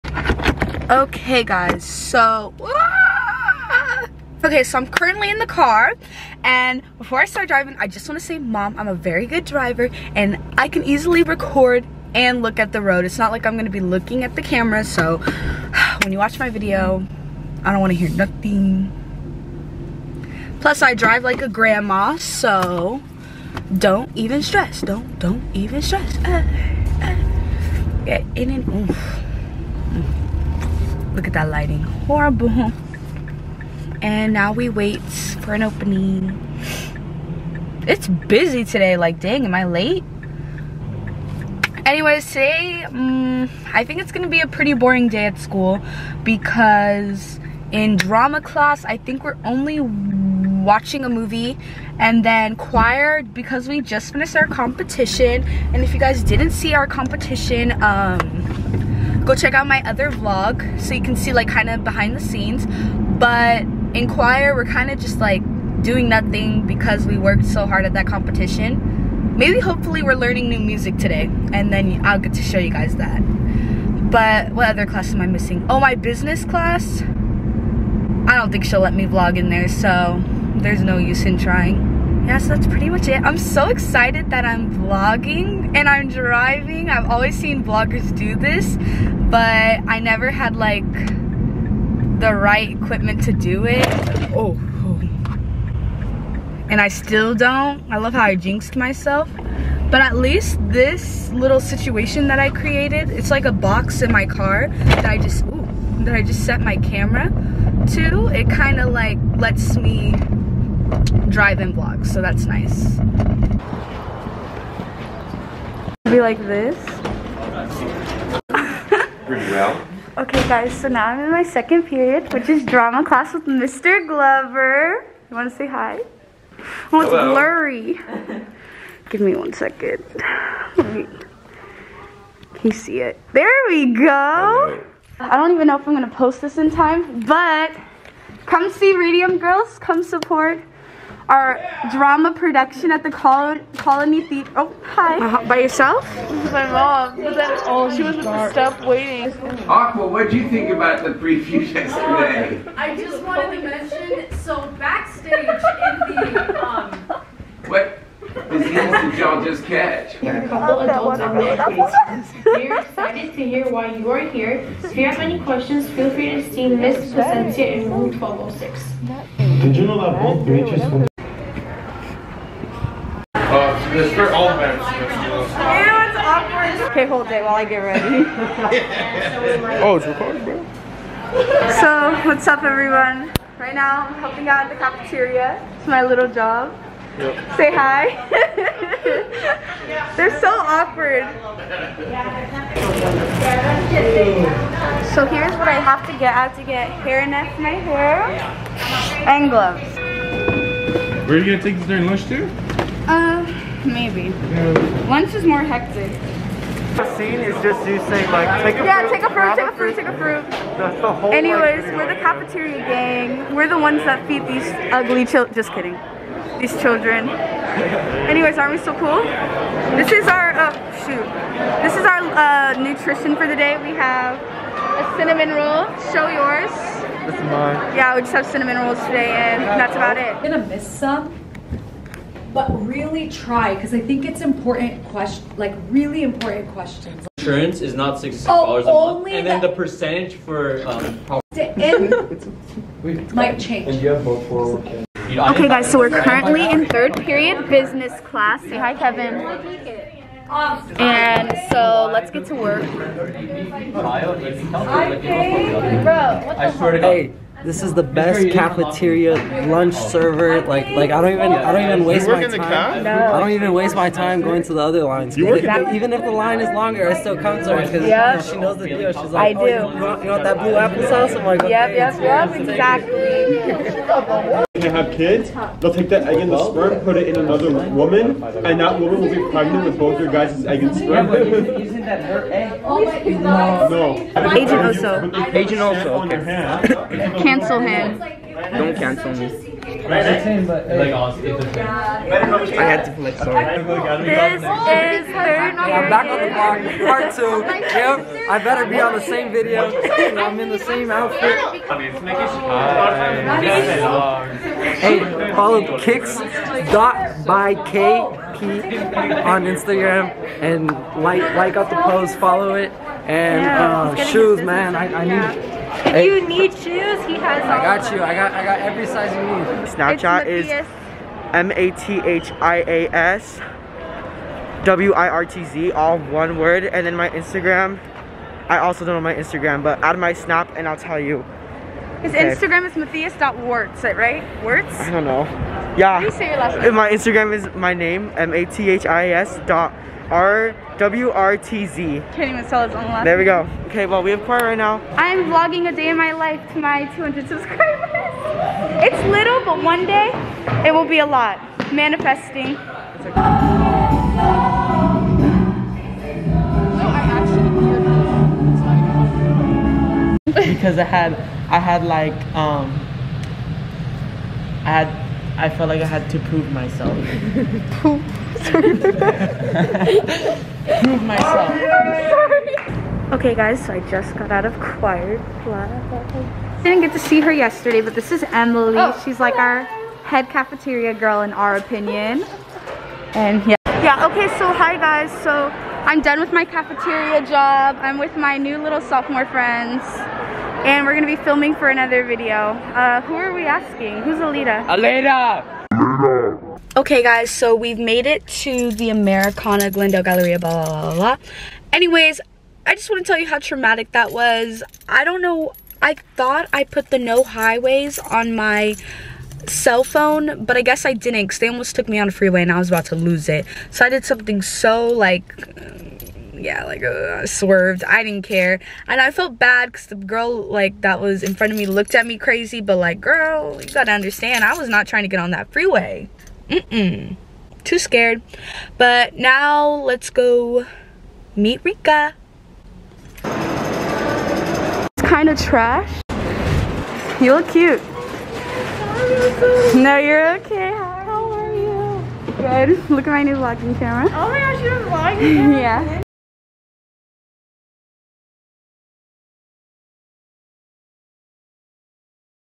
Day in the life of a Junior (I only have 3 classes!!)
You may be wondering why I only have 3 classes and here is why:
I am currently an ILA student who takes my 3 electives at in person school and then comes home to complete my other work. I tried this system out my second semester of junior year and realized that its not for me. With that being said i'm going to transfer back to my original school for Senior year :)

«Be sure to also follow my business instagram (Coming Soon)

«Sub count:214

Age: 17
Sign: Capricorn
Grade: 11th
Birthday: December 27th
tell me a little bit about yourself in the comments